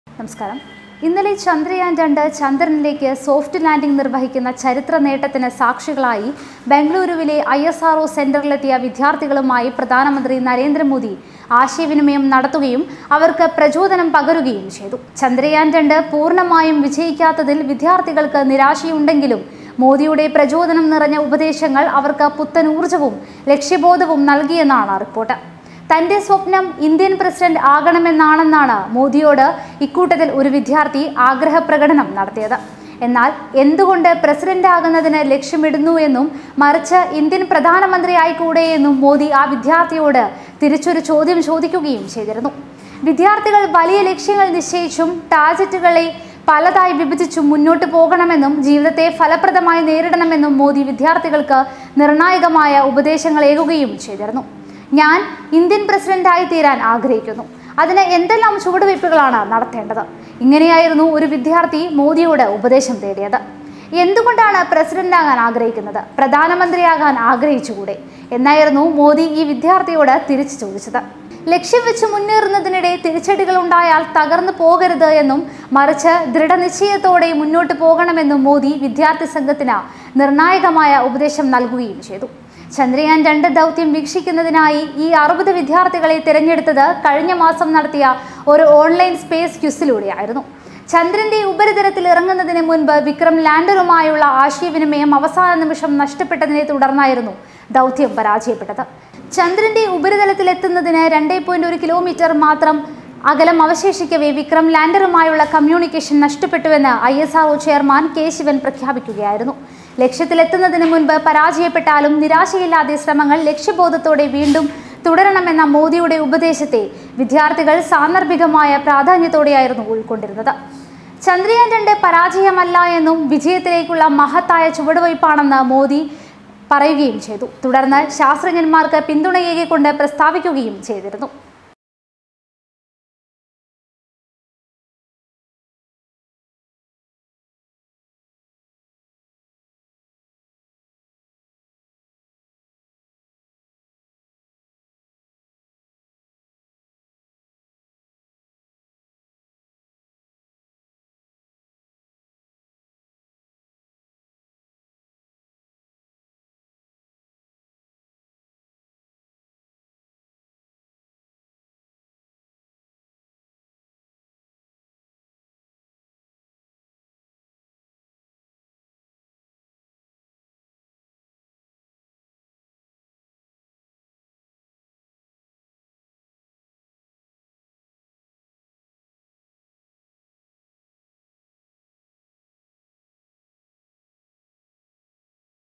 ISRO യില്‍ വിദ്യാര്‍ത്ഥിയുടെ ചോദ്യത്തിന് പ്രധാനമന്ത്രിയുടെ മറുപടി ഇങ്ങനെ I Narendra modi
എന്റെ സ്വപ്നം ഇന്ത്യയുടെ പ്രസിഡന്റ് ആകണമെന്നാണ്; അതിന് ഞാൻ എന്തു ചെയ്യണം? അർദ്ധരാത്രിയിൽ ഇന്ത്യയുടെ ചരിത്ര നേട്ടത്തിന് സാക്ഷികളായി എത്തിയ വിദ്യാർത്ഥികളിൽ ഒരാൾ മോദിയോട് ചോദിച്ചത്; എന്തുകൊണ്ട് പ്രസിഡന്റ്? പ്രധാനമന്ത്രിയായിക്കൂടെ എന്ന് തിരിച്ച് ചോദിച്ച് മോദി; വലിയ ലക്ഷ്യങ്ങൾ നിശ്ചയിച്ചും ടാർജറ്റുകൾ പലതായി വിഭജിച്ചും ജീവിതത്തെ നേരിടേണ്ടത് വിശദീകരിച്ച് മോദി